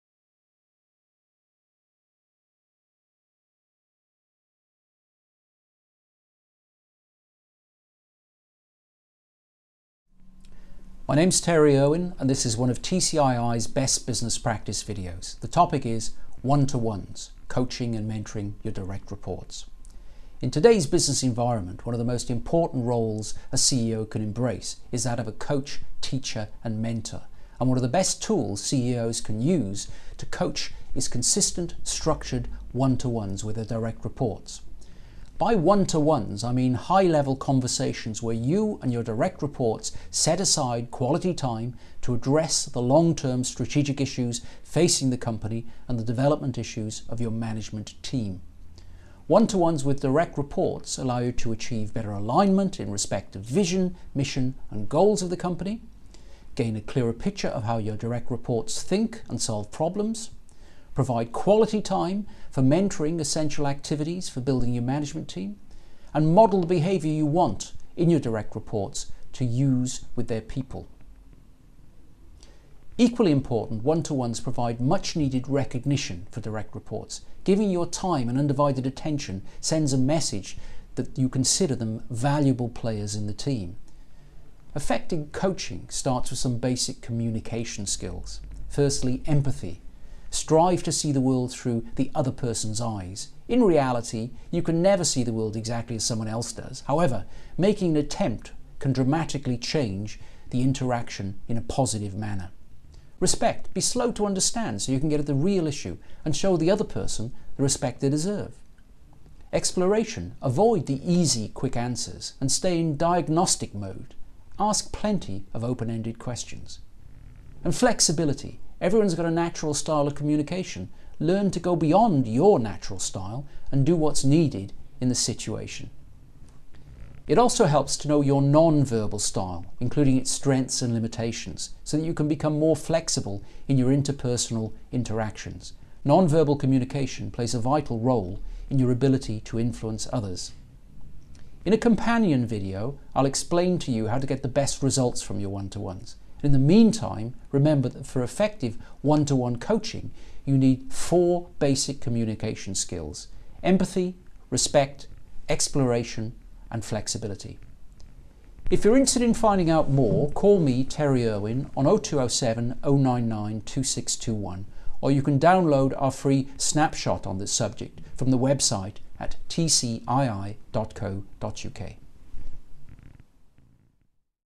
One-to-ones: coaching and mentoring your direct reports
One of the most important roles CEOs can embrace is that of coach, teacher and mentor, using consistent, structured one-to-ones with their direct reports. A TCii Best Business Practice video.